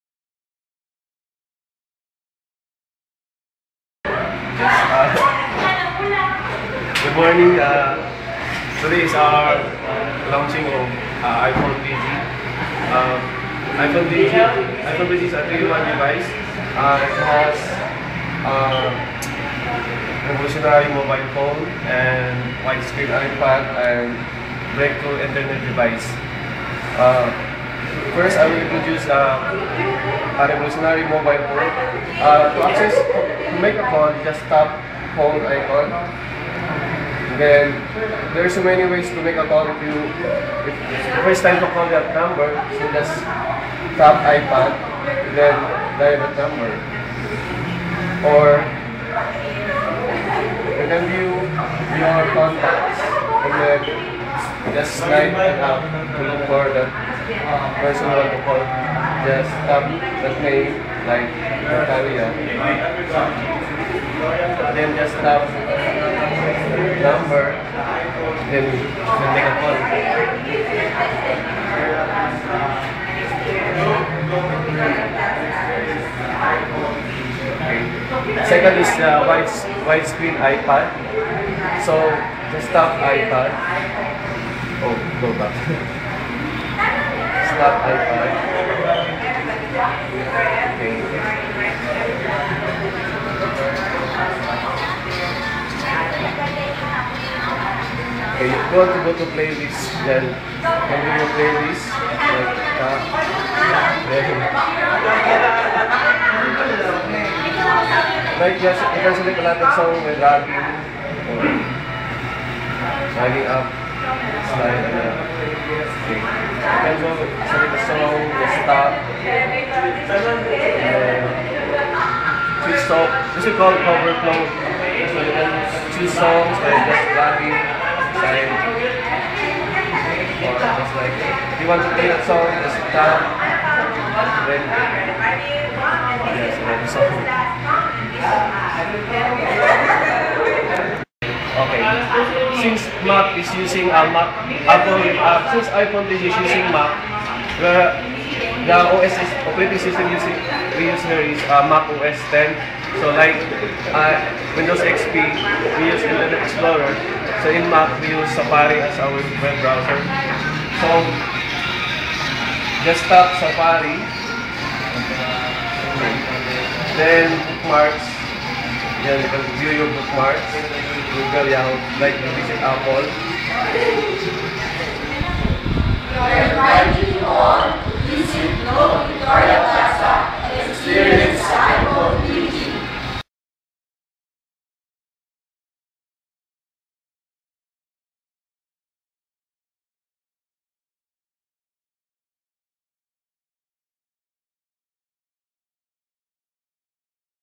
iPhone 3G
Here's a quick look at the iPhone 3G. Thanks to Globe Business Center Victoria Mall, Davao.